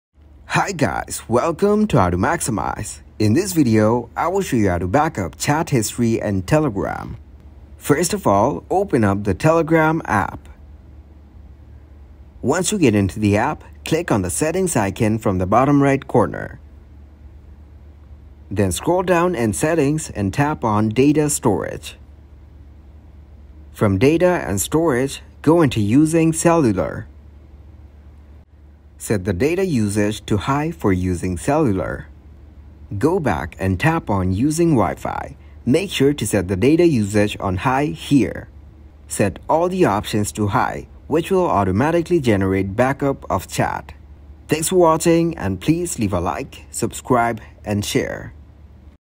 How To Backup Chat History in Telegram (New 2023)
In this tutorial, you will learn How To Backup Chat History in Telegram  in easy steps by following this super helpful tutorial to get a solution to your problem!!